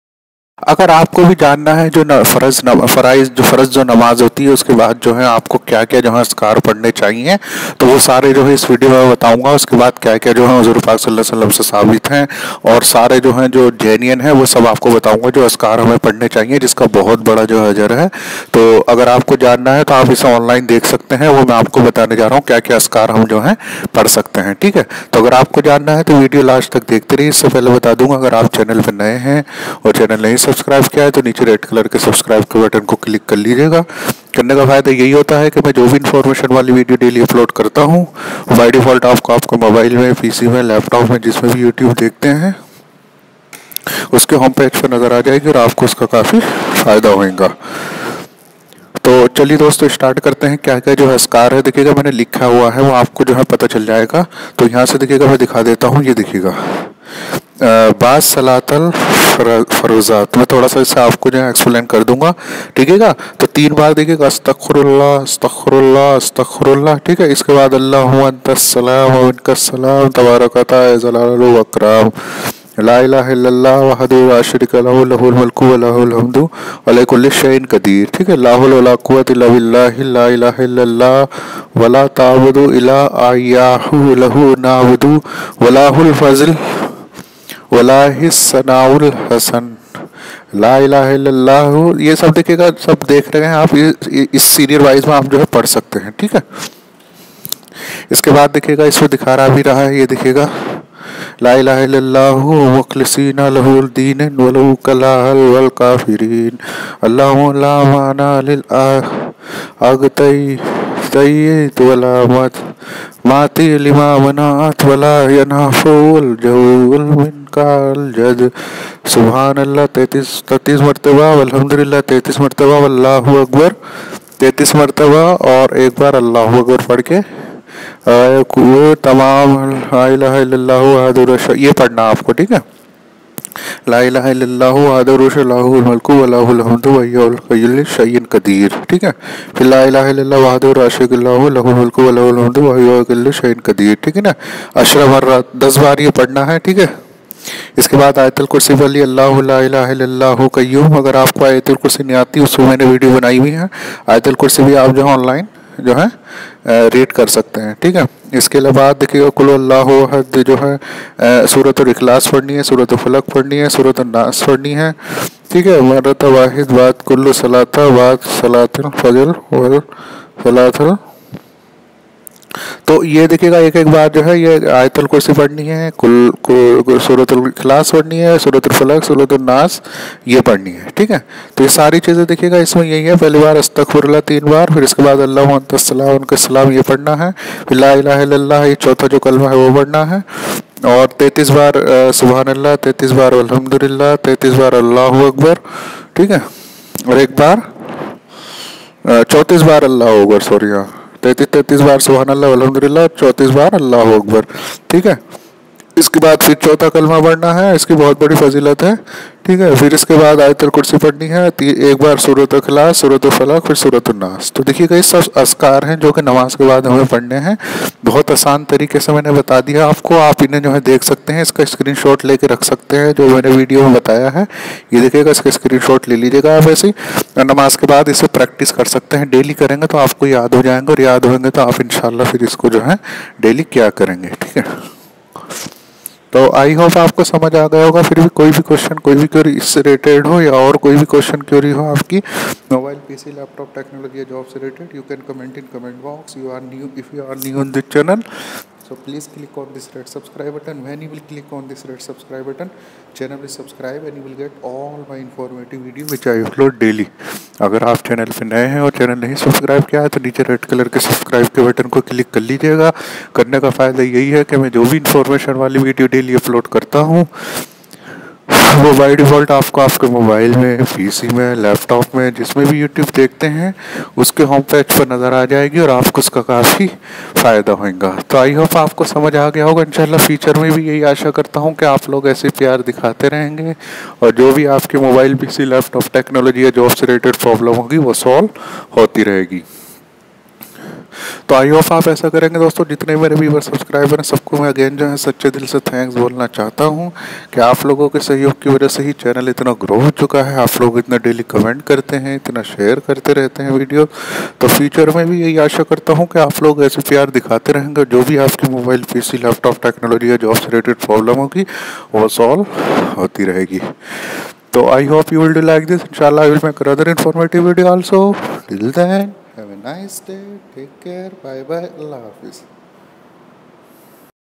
Zohar/Asar/Isha 5 Namaz Farz Ke Bad Dua Ka Tarika
In This Video You Will Find The What We Have To Do Dua After 5 times Prayofislam - Learn Free